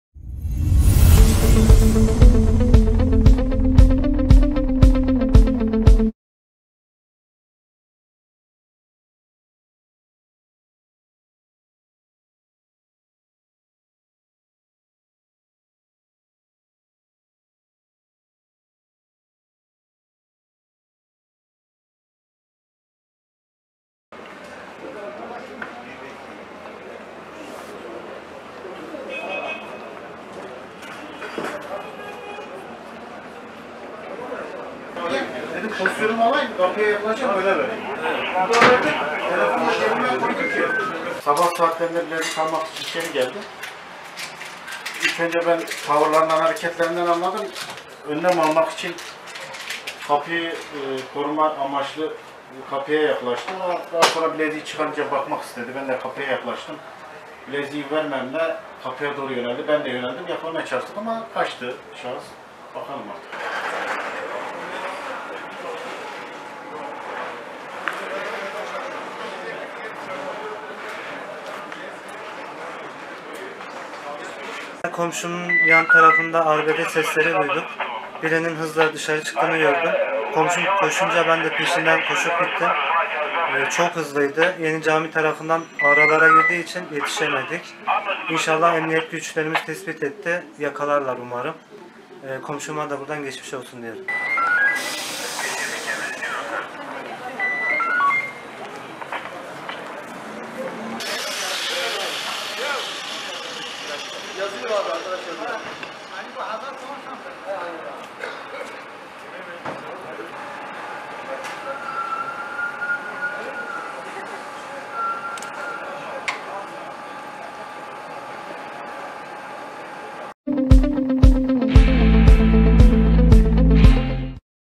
Kuyumcuda ışık hızında kapkaç Esnaftan kaçtı ama kameralara yakalandı
Malatya’da bir kuyumcuda satın alma bahanesiyle incelediği altın bileziği ışık hızıyla alıp kaçan şahsın hızına kimse yetişemedi. Esnaftan kaçmayı başaran kapkaç zanlısı kameralardan kaçamadı. Olay, sabah 10.30 sularından Battalgazi ilçesine bağlı Mahallesi Yenihamam Mahallesi’nde meydana geldi.